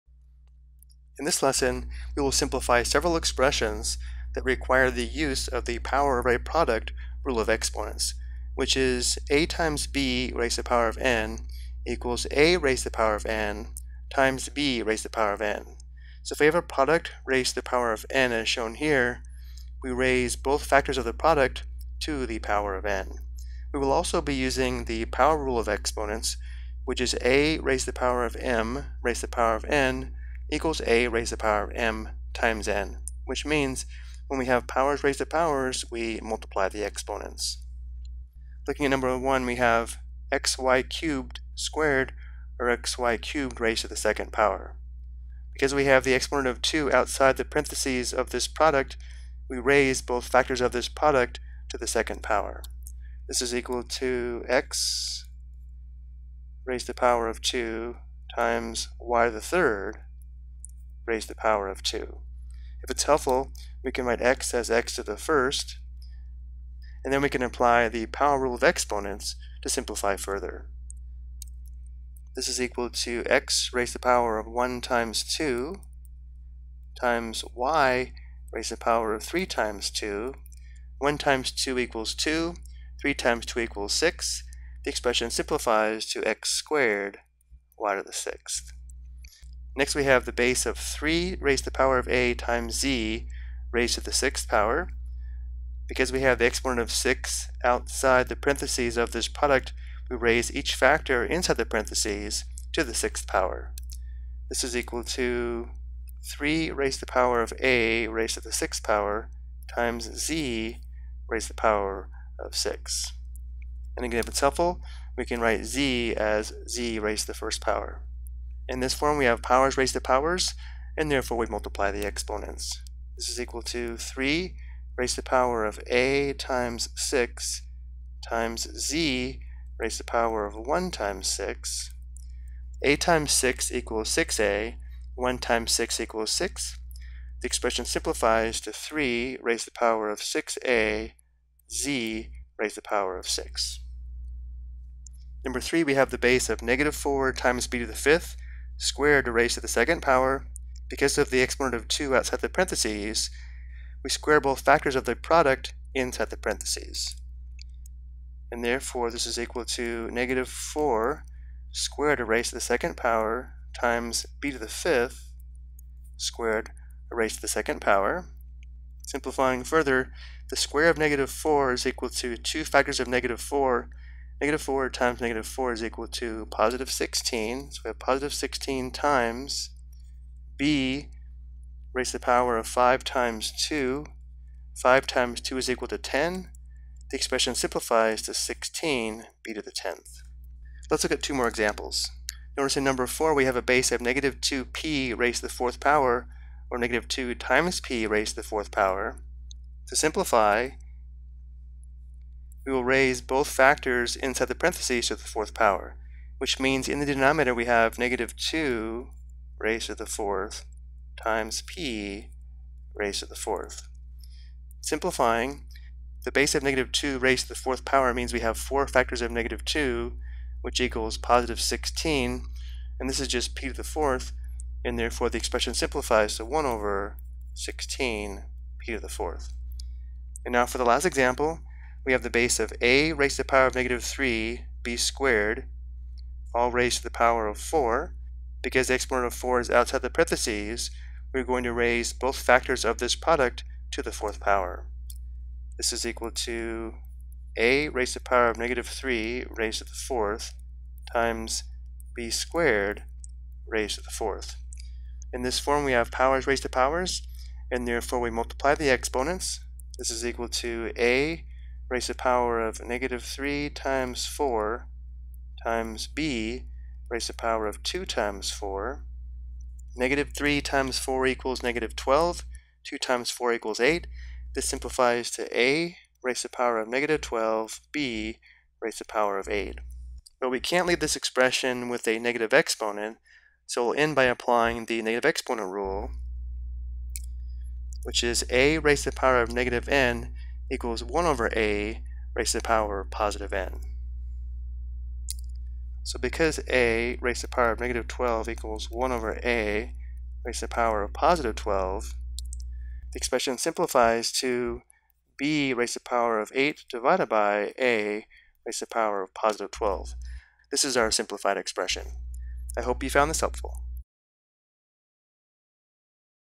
Simplify Expressions Using Exponent Rules (Power of a Product)
This video explains how to simplify exponential expressions using exponent rules.